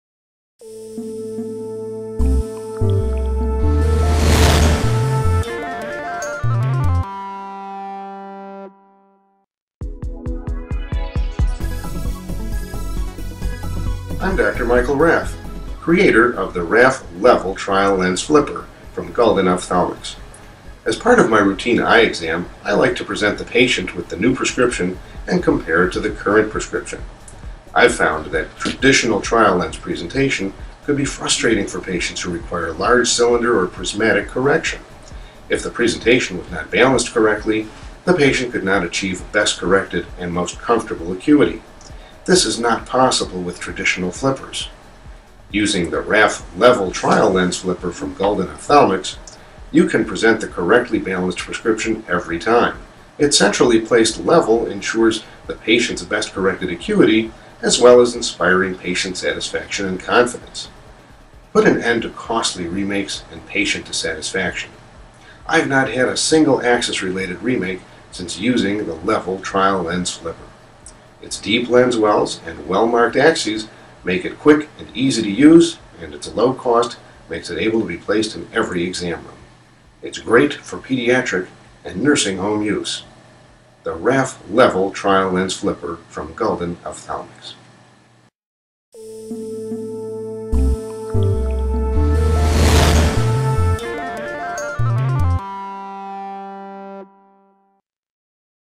Raff Level Trial Flipper
The Raff Level Trial Lens flipper balances lenses with a level eliminate off axis rotation with ease. Balance prescriptions with a level to eliminate off axis rotation. Eliminate costly remakes with a more accurate trial lens presentation. Inspire patient confidence by providing a positive visual experience. Use to balance cylinder and prismatic corrections. Quick and easy to use. Great for pediatric and nursing home use. Its low cost makes it affordable to outfit multiple treatment rooms in eye care offices with this flipper.